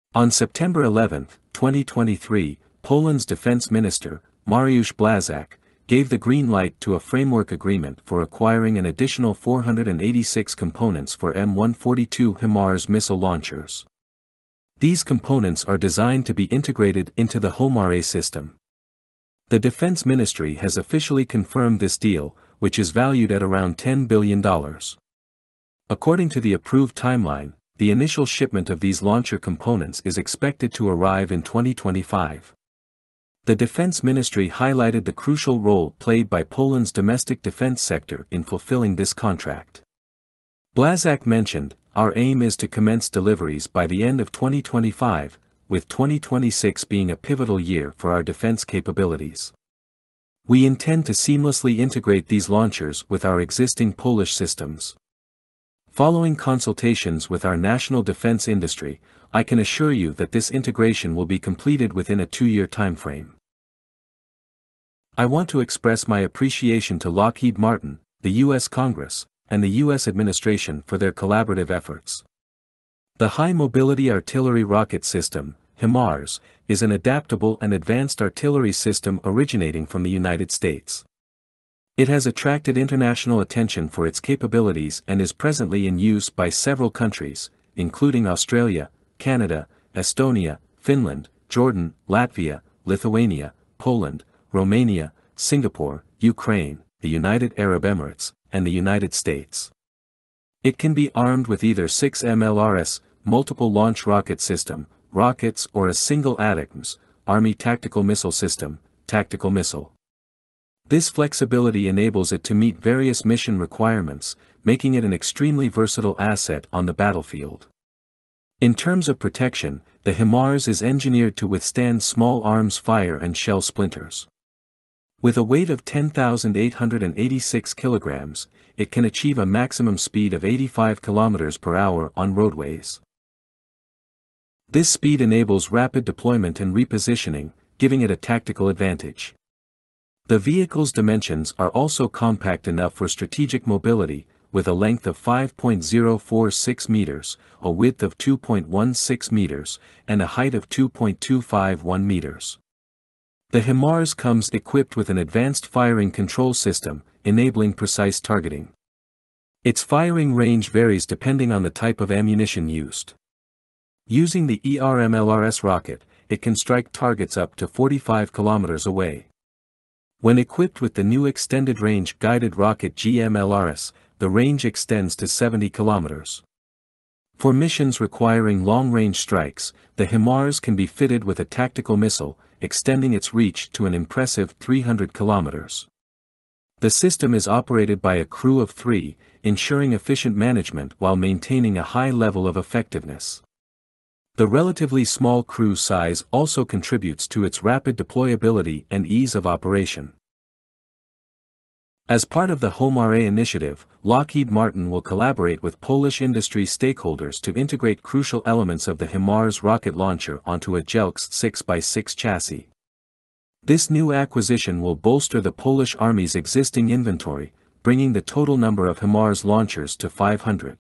Poland signs USD 10 Bn contract for 486 M142 HIMARS MLRS
On September 11, 2023, Mariusz Błaszczak, Poland's Defense Minister, approved a framework agreement for the purchase of 486 additional M142 HIMARS missile launcher components, designed to be integrated into the HOMAR-A system. The Defense Ministry confirmed the deal, valued at approximately $10 billion.